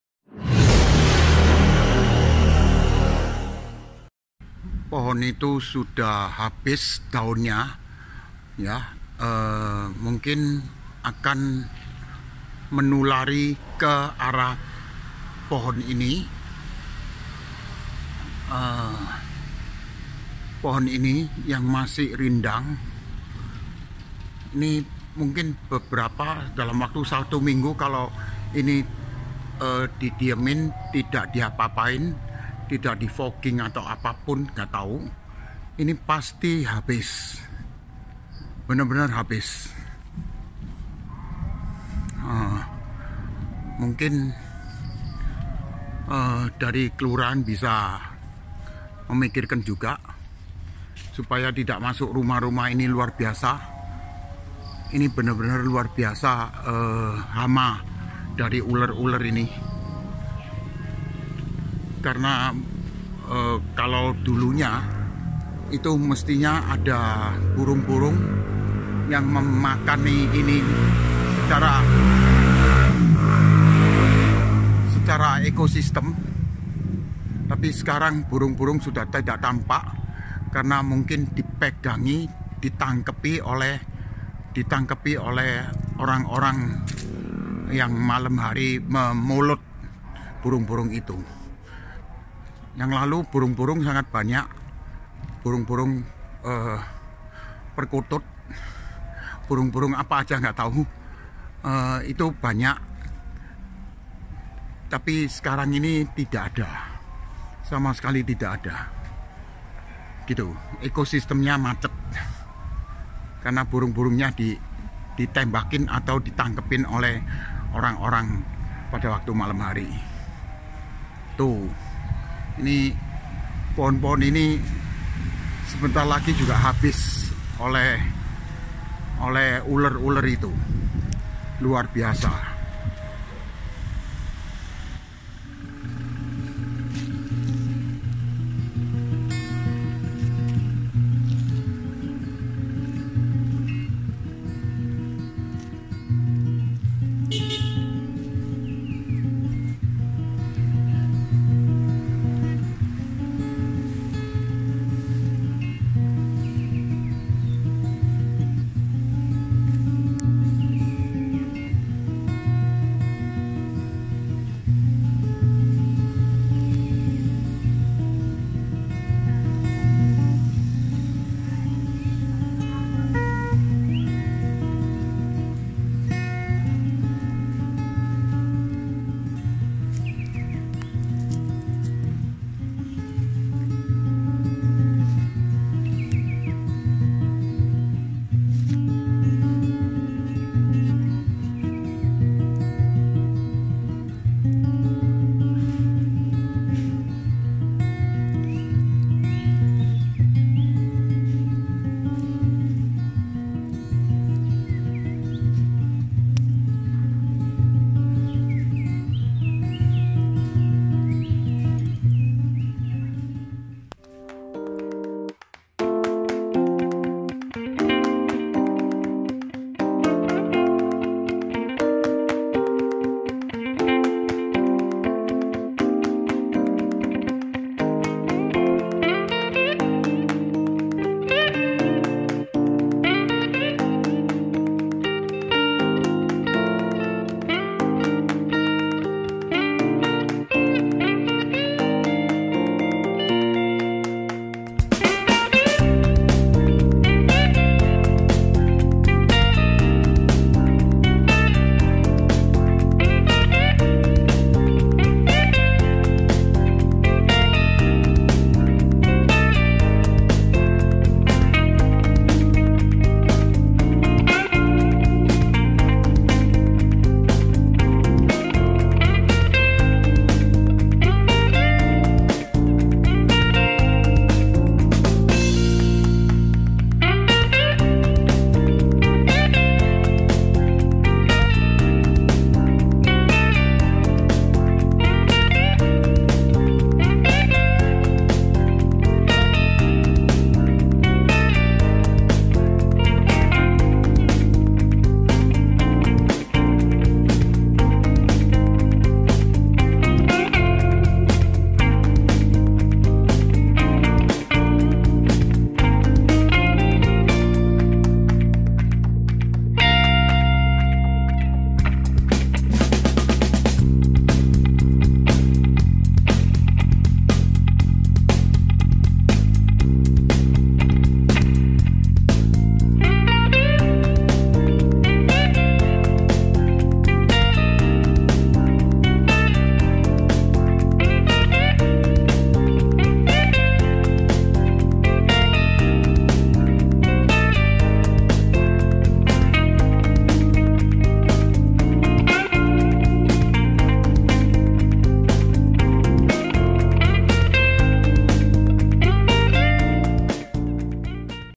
Caterpillar outbreak!!!
Caterpillar outbreak in my hometown.. !!